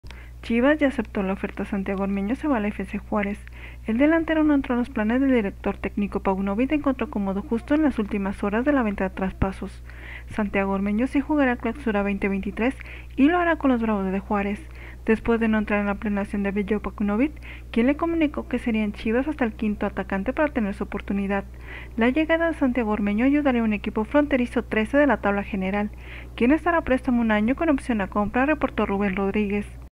"SANTIAGO ORMEÑO FRONTERIZO"
El delantero no entró en los planes de director tecnico  Paunovic y encontró acomodo justo en las últimas horas de la ventana de traspasos...

Santiago Ormeño sí jugará en el Clausura 2023 y lo hará con los bravos de Juárez, después de no entrar en la planeación de Veljko Paunovic, quien le comunicó que sería en Chivas hasta el quinto atacante para tener su oportunidad.

La llegada de Santiago Ormeño ayudaría a un equipo fronterizo 13 de la tabla general, quien estará a préstamo un año con opción a compra, reportó Rubén Rodríguez.

"Es oficial Santiago Ormeño viajo Esta noche  a Juárez para mañana
aplicarse exámenes médicos”.

 Semanas atrás, en se dio a conocer la determinación del cuerpo técnico del Rebaño, que dejó sin opción de pelear al también seleccionado peruano, a pesar de que fue el goleador de la pretemporada.

“Veljko Paunovic llegó al entrenamiento y le dijo a Santi Ormeño: ‘No entras en planes, confío en Ronaldo Cisneros, (Daniel) Ríos y voy a espera a (José Juan) Macías, así que gracias, vele buscando equipo.

Santiago Ormeño causará baja antes de que acabe el campeonato. Santiago Ormeño tiene tres años de contrato, con Chivas”, compartió en su momento Gustavo Mendoza.

Chivas, además, perdió en la Jornada 2 Alex Vega, quien se lesionó la rodilla derecha contra Atlético de San Luis y se perderá hasta siete semanas por la rehabilitación.

Santiago Ormeño llegó en julio de 2022 como refuerzo del chiverío, después de la baja de José Juan Macías para el arranque del Apertura 2022, una contratación que causó polémica ya que es seleccionado peruano.